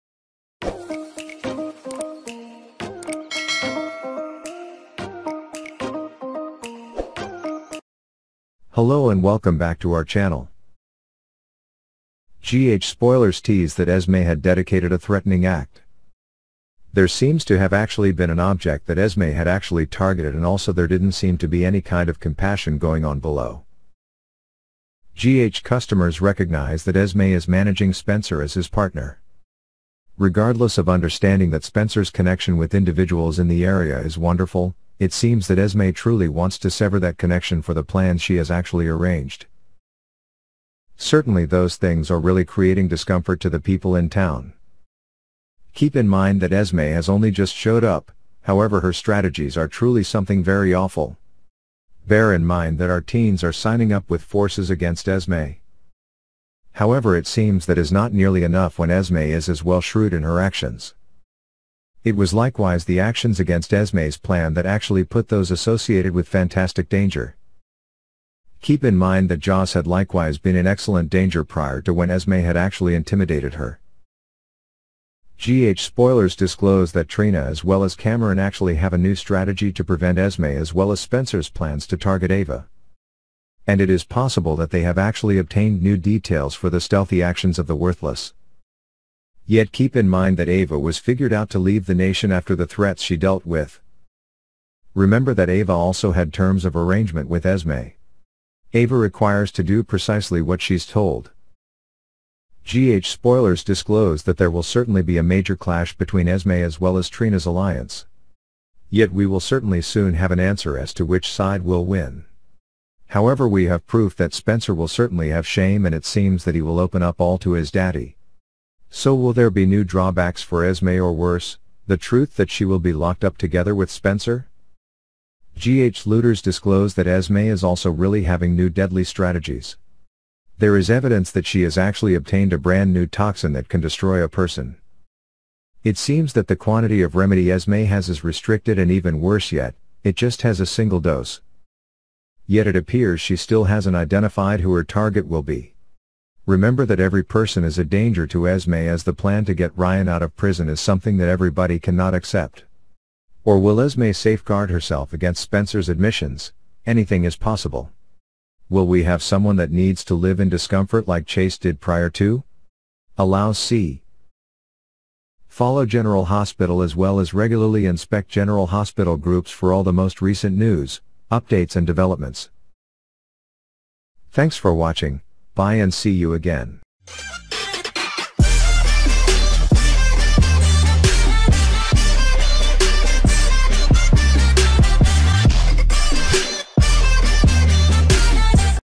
General Hospital Spoilers: Esme executes poisoning after being provoked
General Hospital Spoilers 9/2021:  Esme executes poisoning after being provoked